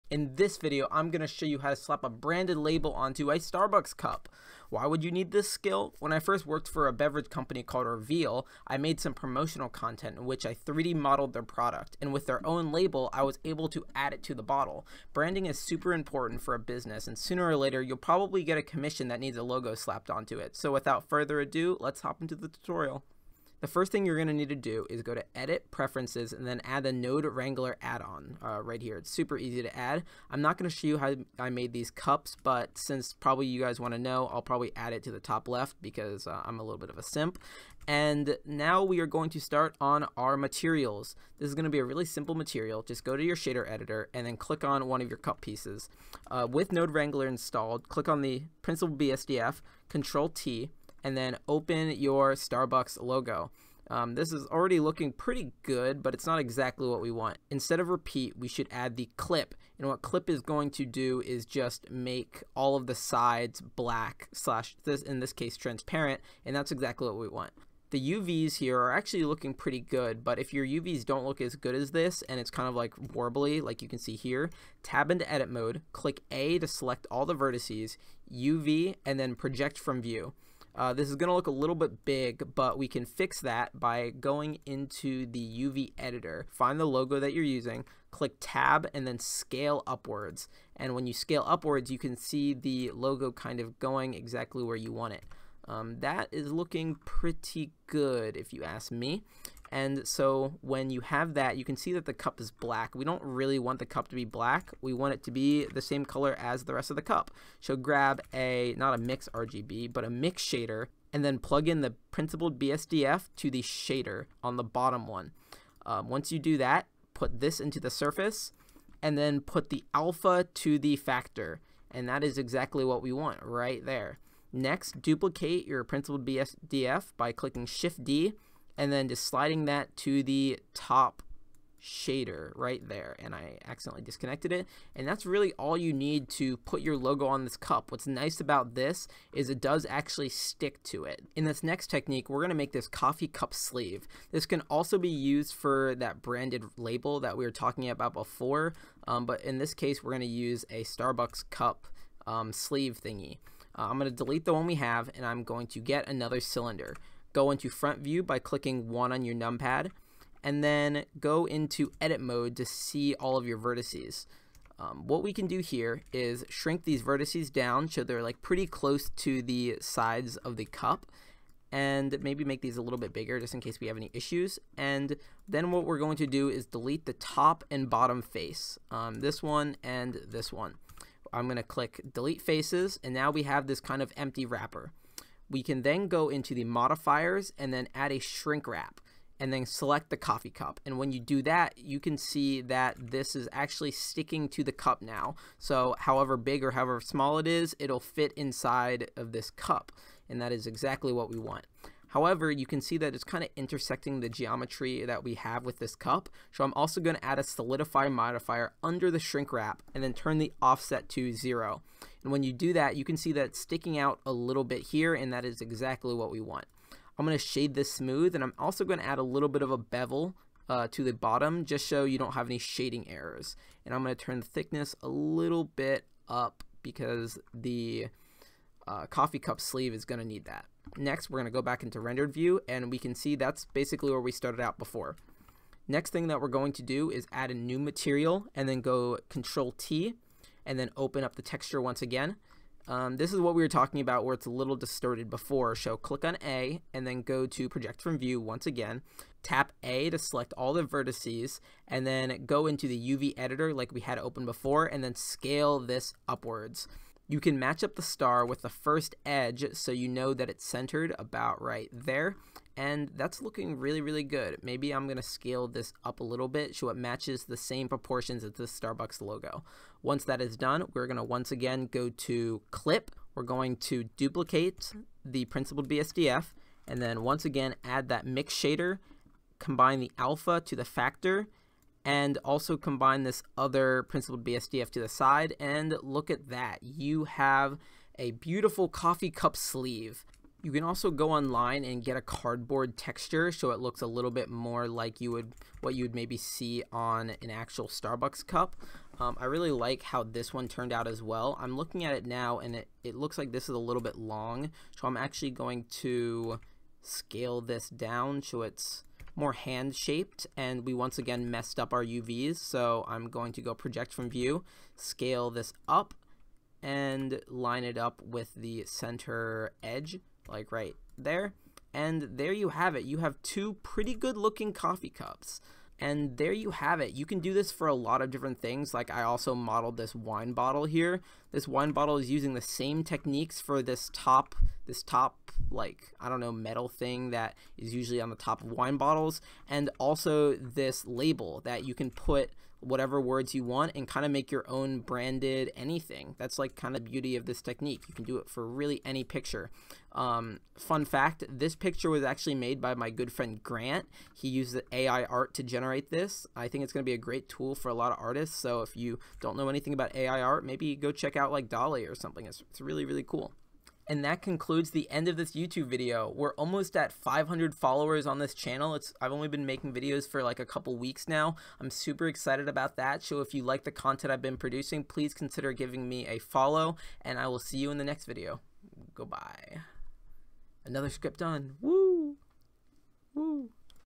Branding ANYTHING with Free software BLENDER [Tutorial]
Thanks SO much for the support on the last video! I am following the same technique for this one just because it did so well. Hope this video helps you brand your own products and if you liked the style, a follow would sure help me produce more content like this :)

Reveal (The first drink made from avocado seeds!)

Timestamps:
0:00 - How to use
0:24 - UV / Image texture
2:22 - Shrink-wrap Coffee sleeve
5:23 - Extras
6:03 - Outro